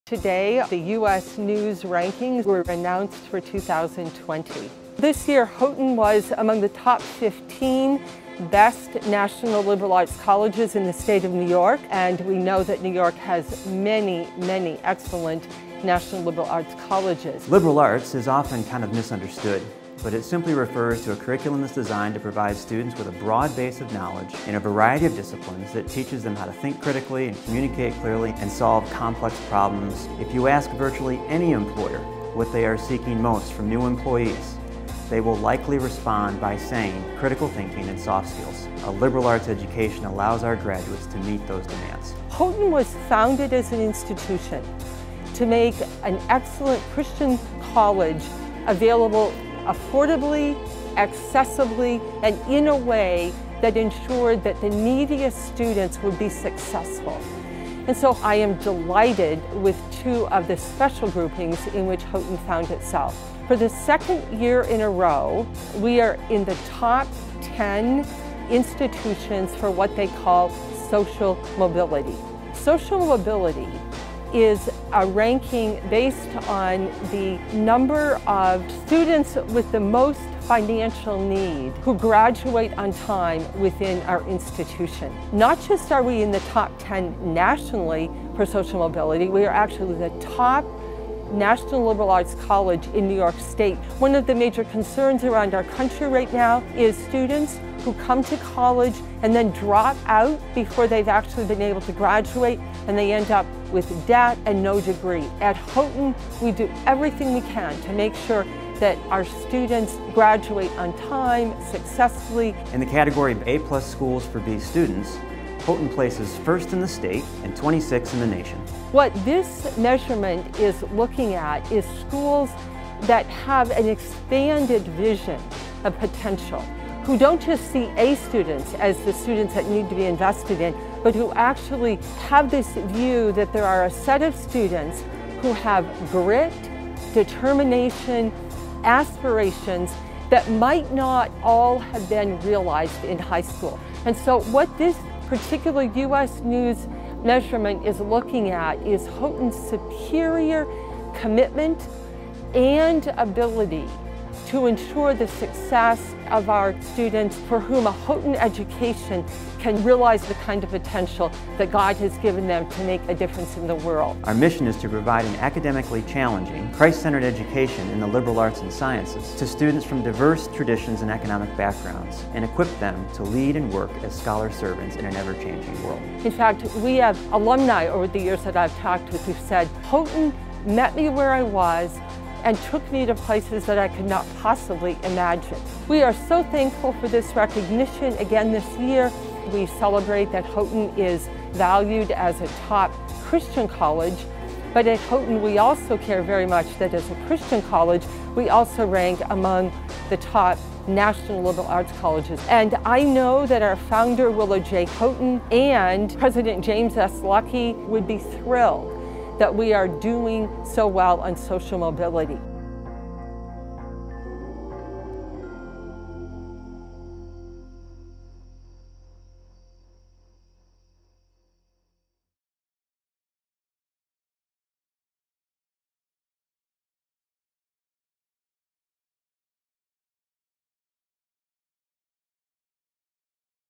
Houghton College Named Best National Liberal Arts College by U.S. News
U.S. News and World Report has named Houghton College the top performer among Best National Liberal Arts Colleges in New York State for Social Mobility. Houghton also claimed the top spot for Social Mobility among schools in the Council for Christian Colleges and Universities (CCCU) and the number 10 spot in the nation.

Houghton College also earned the number #1 spot among Best National Liberal Arts Colleges in New York State for the category “A+ Schools for B students," and was included as a Best National Liberal Arts College for the 11th consecutive year, coming in at #3 among CCCU schools, in the top 15 in New York and 127th in the nation.